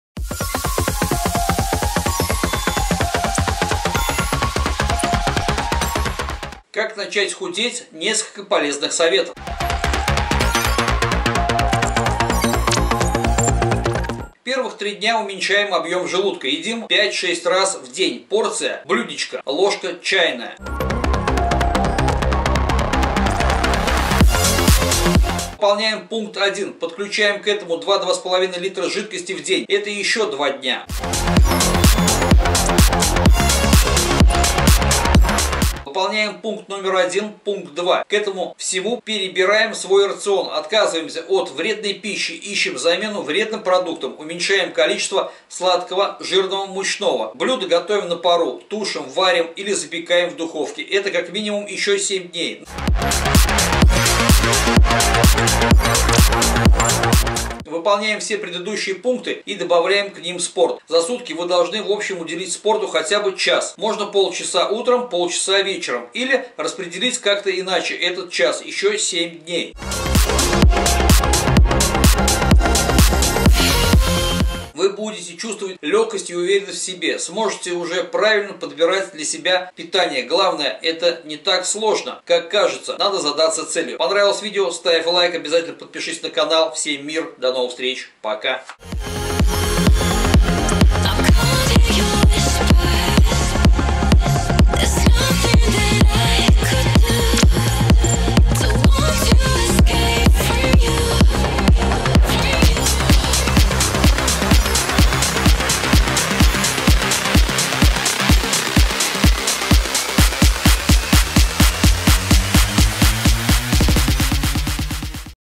КАК НАЧАТЬ ХУДЕТЬ?НЕСКОЛЬКО ПОЛЕЗНЫХ СОВЕТОВ
Муыка: MIDNIGHT CVLT

ПИШИТЕ не СТЕСНЯЙТЕСЬ ВСЕМ ОТВЕЧУ И ПОМОГУ:
1.СОСТАВИТЬ ПРОГРАММУ ТРЕНИРОВОК,
2.СОСТАВИТЬ ПРОГРАММУ ПИТАНИЯ,
3.ПОДБОР СПОРТИВНОГО ПИТАНИЯ,
4.ПЕРСОНАЛЬНЫЕ ONLINE ТРЕНИРОВКИ/ВЕДЕНИЕ 24/7